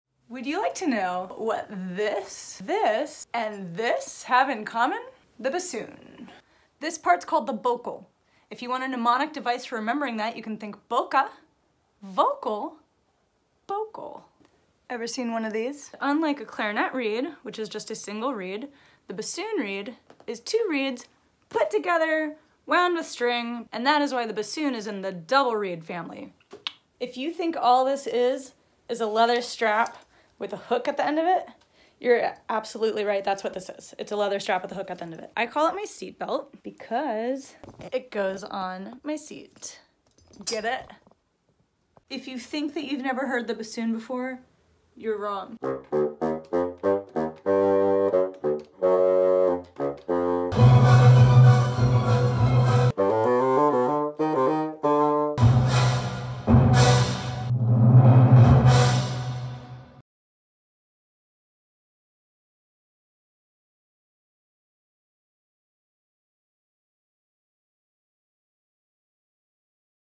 Learning With La Louma #2: Bassoon (musical instrument series)
Ready to learn about the bassoon?? Welcome to Video #2 of "Learning with La Louma", a new series of short, nerdy videos to be released Mondays, where I share the different instruments I play. Buckle up because the list is long!
Here's some vulnerable honesty...
I have an emotionally complex relationship with my bassoon. Truth: I immediately started crying just now, simply from typing that. Having started on flute, my musical world always revolved around lead melodies, so the bassoon taught me about bass lines, countermelodies, and harmonies, and of course gave me a reason to get fluent in reading bass clef (I still think in terms of bassoon when I read bass clef now, just like I think in terms of flute when reading treble clef). I just sighed. The bassoon. I love the way my face kind of rattles a little bit when I play a low Bb. The vibrations! So cool! And I love how my eyebrows instinctively go into a look of concern and care when playing in the falsetto range — it feels so incredibly intimate and vulnerable being up there. But that damn bassoon. It wrecked my hands. The demand on one’s thumbs and pinkies in particular is just ridiculous, especially if you don’t have massive hands (note: I don’t have massive hands). Bassoon was my primary instrument in college, and as soon as I graduated I had to quit playing music entirely, because my hands were in such extraordinary pain and had grown weak and tired. So I entered the music business and didn’t play music for about 8 years, starting again only about 4 years ago. (from my “Backline” interview with CASH Music’s Watt site)